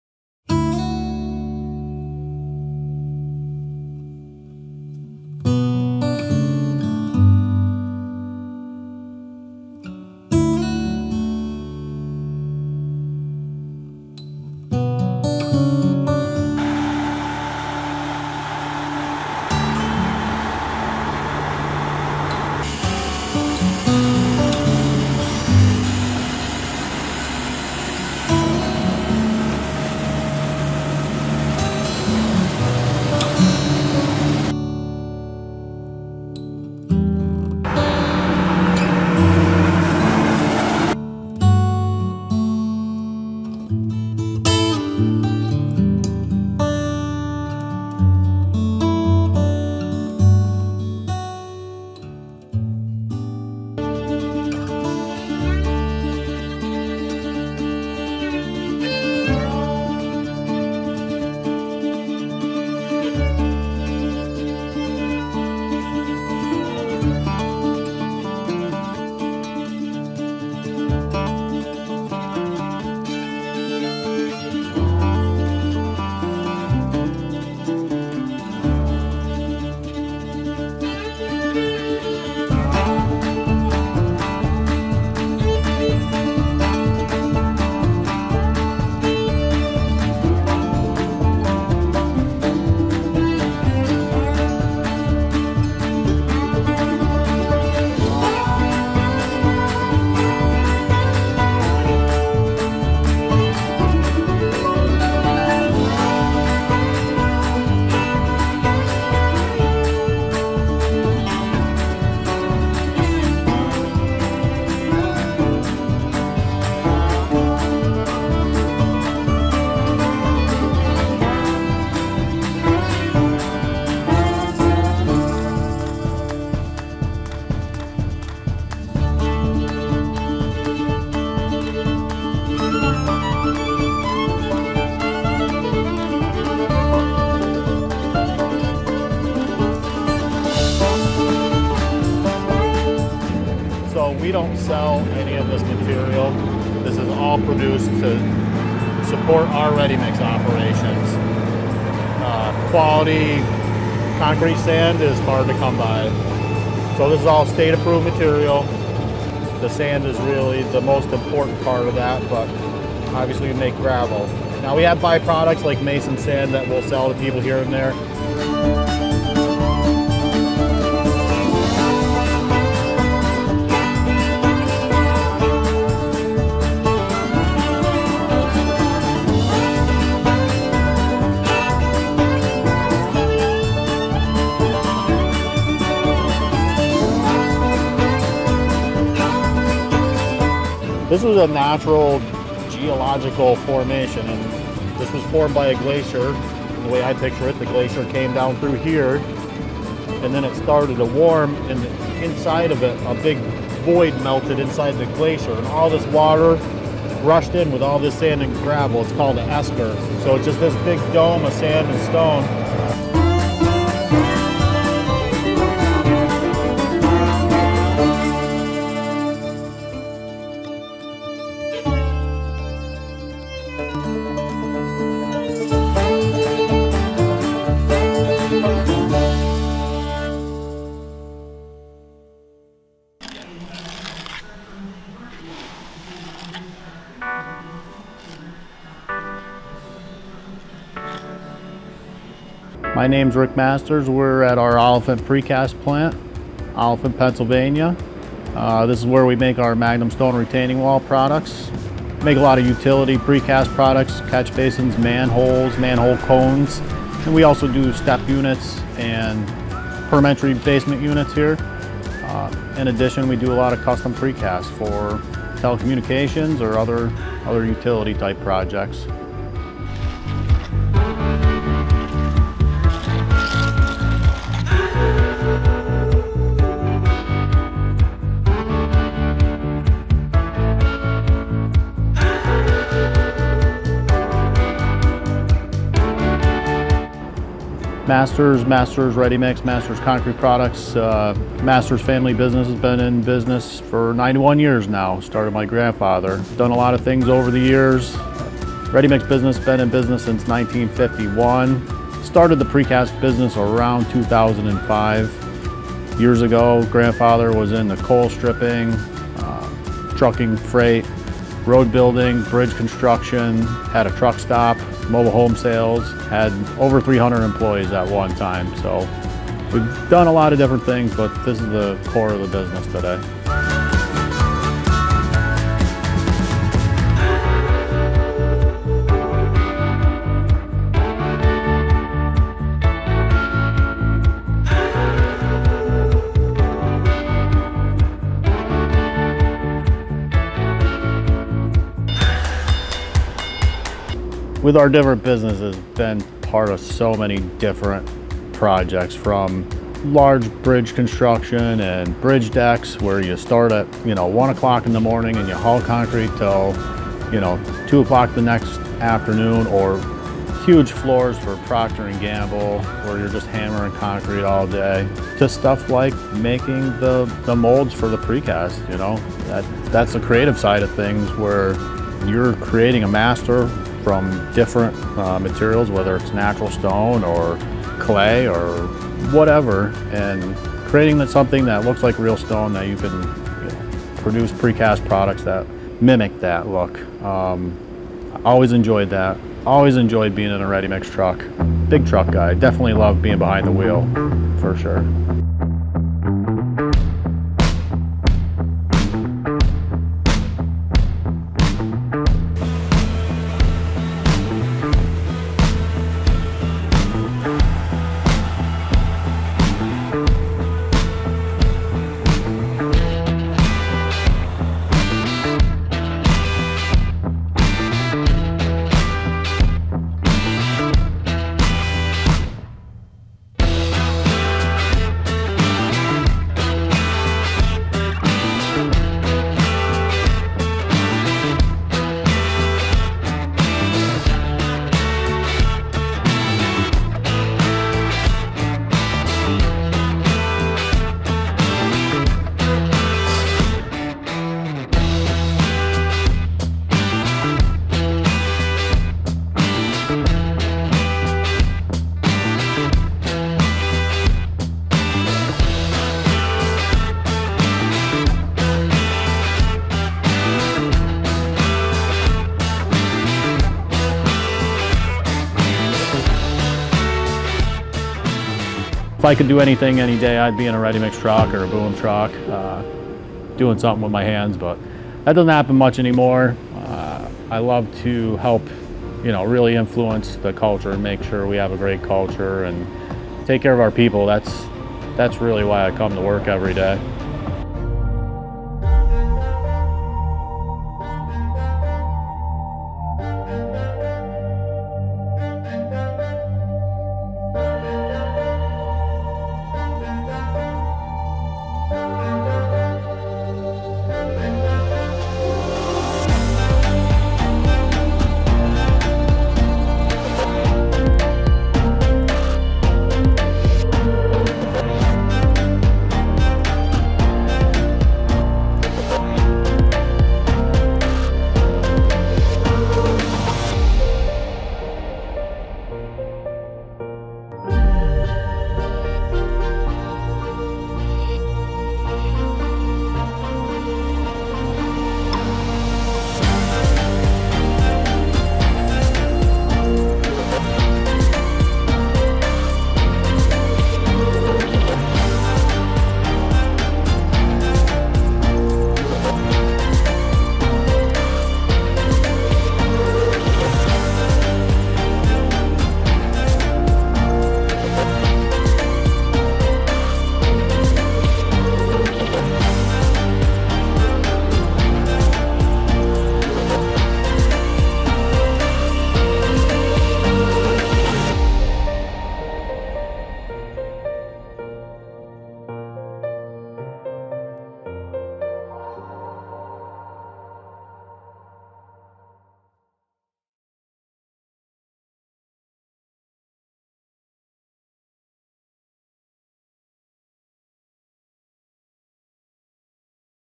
Masters Concrete
Masters Ready-mix, Masters Pre-Cast, Masters Magnum Stone Products...Seeing a trend here? 91 years in business! Check out just a few of their amazing operations as Rick Masters gives us a tour and the history of his family's legacy. We get the impression there is definitely another 91 years ahead. It was a pleasure documenting and capturing yet another Northeast Pennsylvania operation just absolutely crushing it.